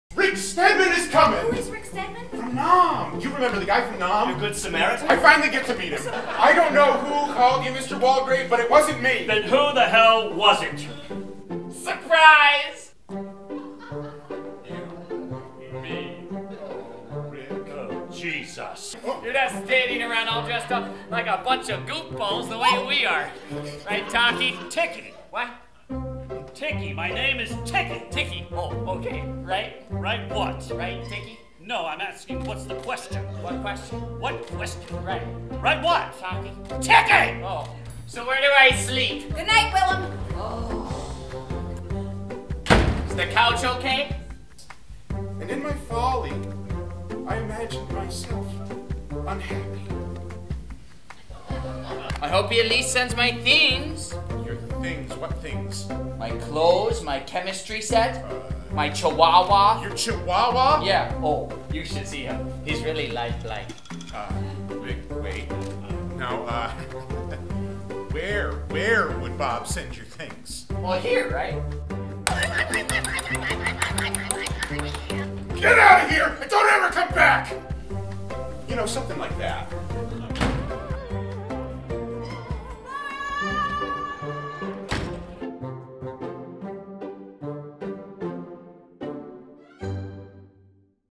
"The Nerd" Trailer | Milwaukee Rep
From the much-loved playwright of The Foreigner comes a new production of one of the funniest plays ever written.

When Willum has an unexpected party guest, who turns into an unwanted houseguest, he executes an elaborate plan to rid himself of the wacky nuisance. Aided by a rag-tag team that includes friends, a would-be lover and an oblivious boss, creative acts of desperation quickly dissolve into utter mayhem. The twists and turns of this madcap comedy lead to an ending that leaves you feeling happily hoodwinked!